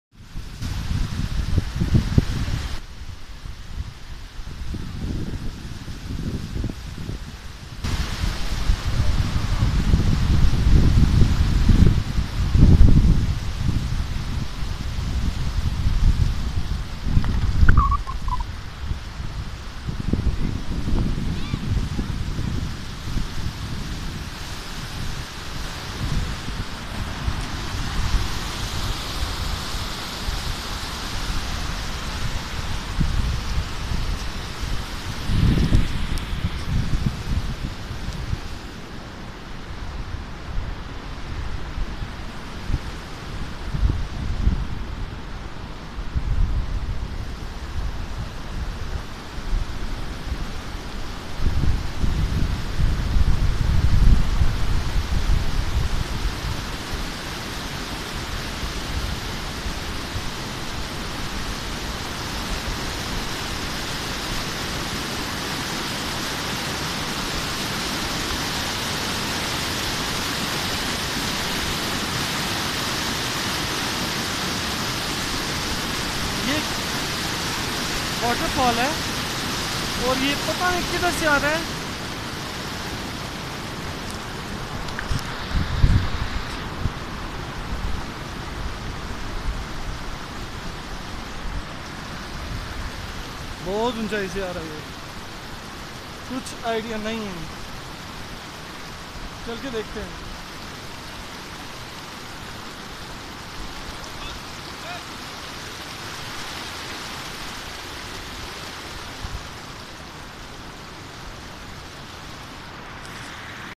mjjupiter (MooN Butt) babu sar top Naran
Babusar Pass
Mountain pass in Pakistan
Babusar Pass or Babusar Top is a mountain pass at the north of the 150 km. long Kaghan Valley connecting it via the Thak Nala with Chilas on the Karakoram Highway. It is the highest point in the Kaghan Valley that can be easily accessed by cars. Wikipedia